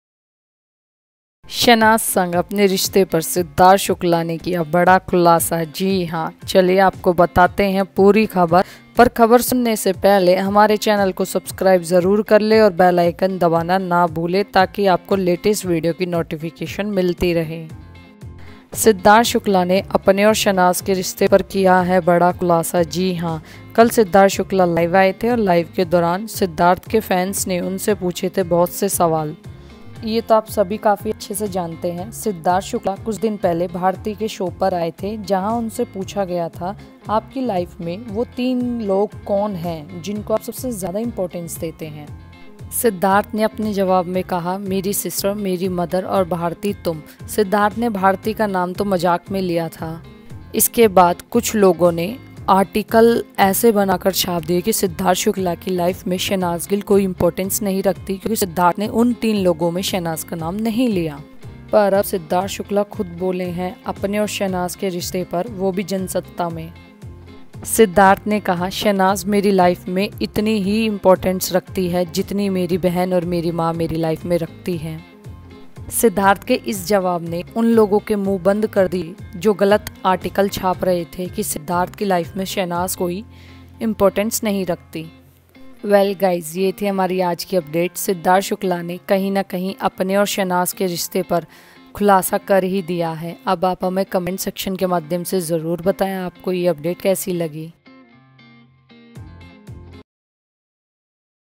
Siddharth Shukla ने Shehnaaz Gill के साथ अपने रिश्ते पर किया बड़ा खुलासा | Celebs Samachar
Note: All the images/pictures shown in the video belong to the respected owners. I am no the owner of any pictures showed in the video.